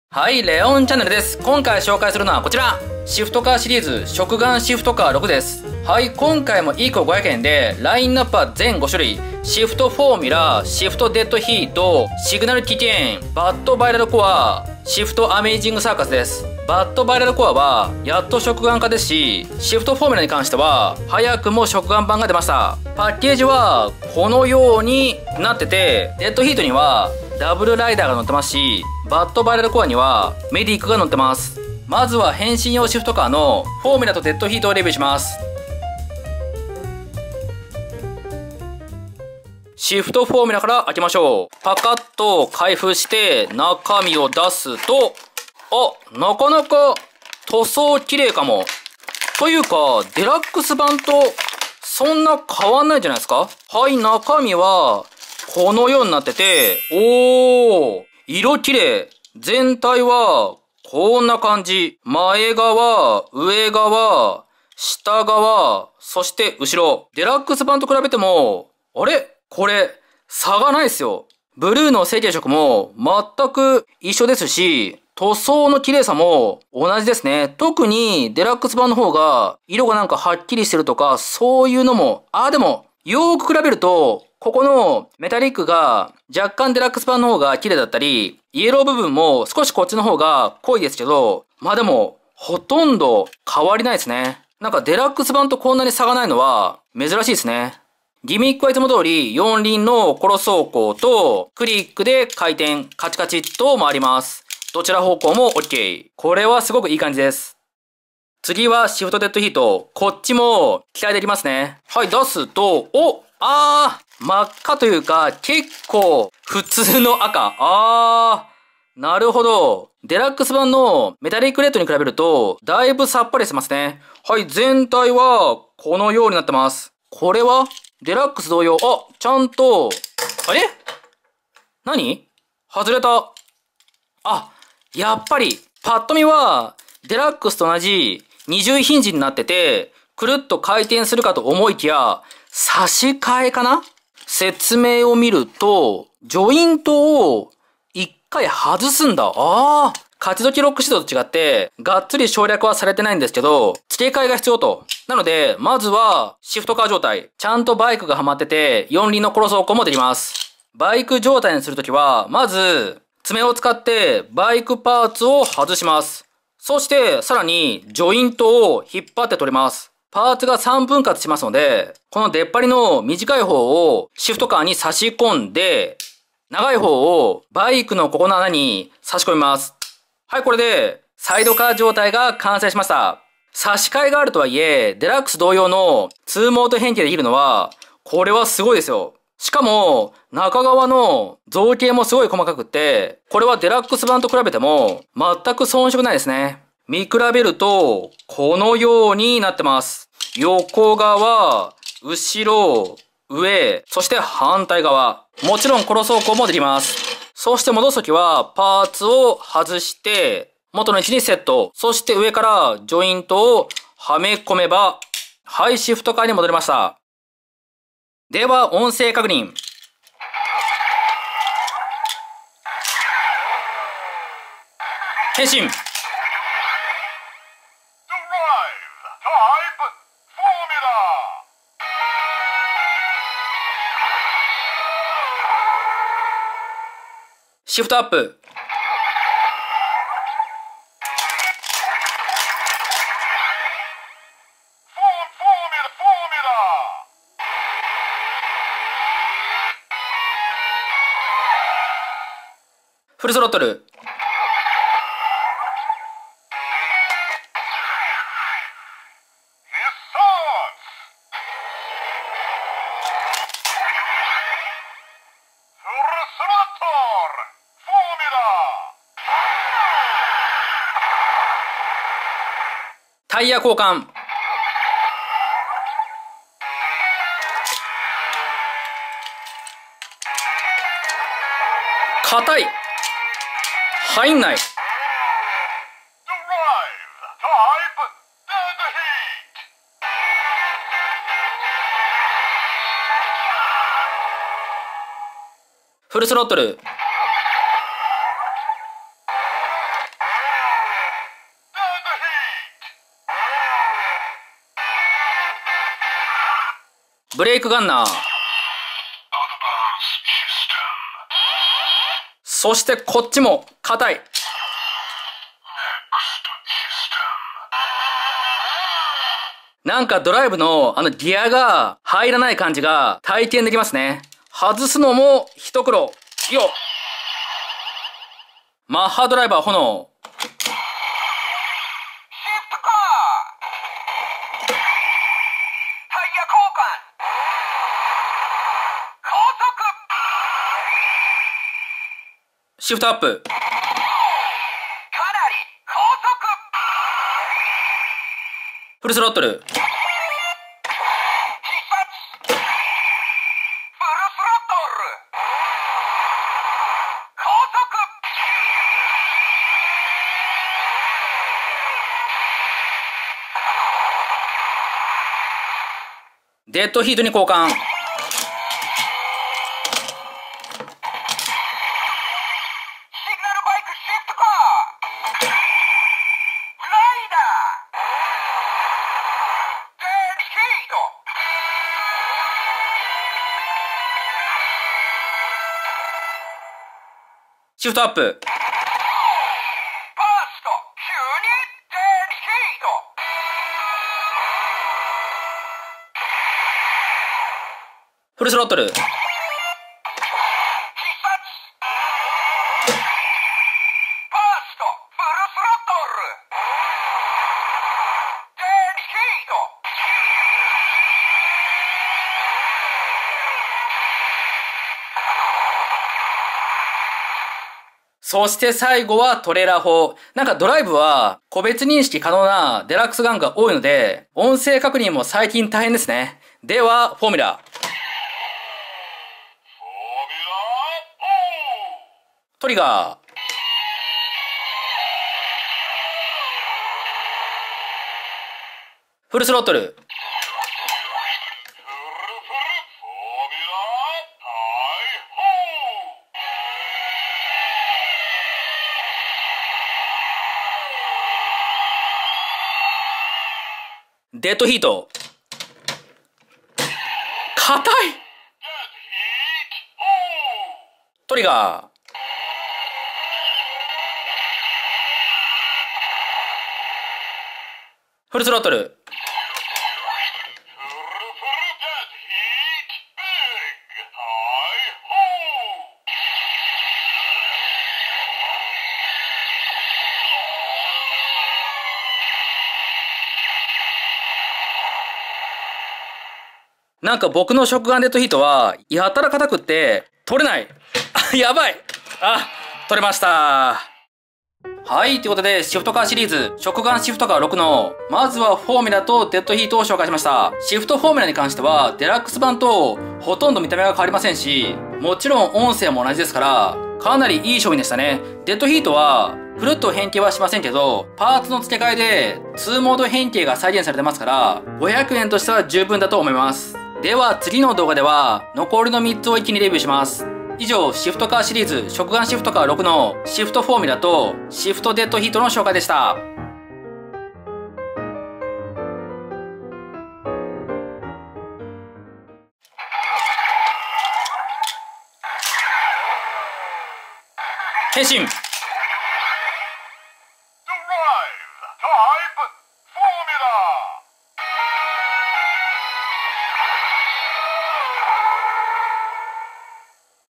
食玩版シフトフォーミュラが発売！SGシフトカー6 フォーミュラ & デッドヒート レビュー！全5種 バット・キケーン・サーカス 仮面ライダードライブ
食玩SGシフトカー動画シリーズ
SGシフトカー6 全5種
SGシフトカー4 全6種
SGシフトカー3 全6種
SGシフトカー2 全5種
食玩SGシフトカー1 全5種